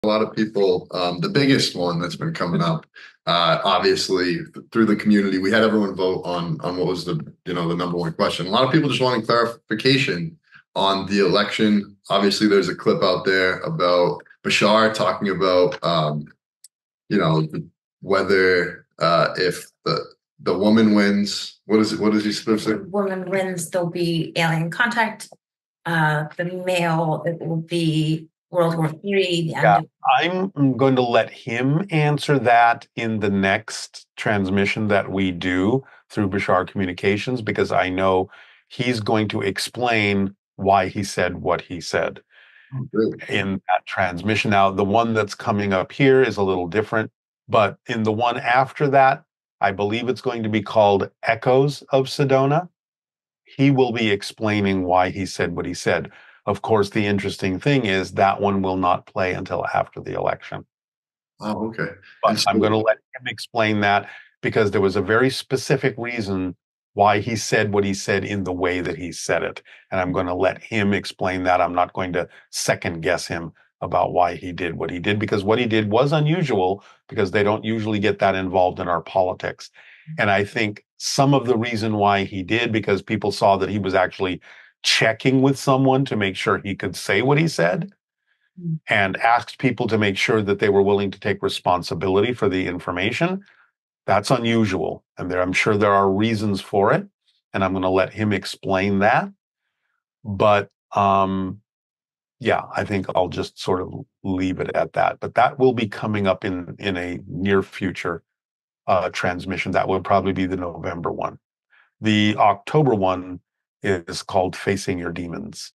Update on the election! 🇺🇸
Darryl Anka explains further what Bashar meant when he talked about the 2024 US presidential election!

His original message seemed a bit cryptic , here is a clip of it:

I am glad Darryl was able to explain a bit further!  Here is the full interview:

Andrea Quagenti from aquaroselife made this clip ✨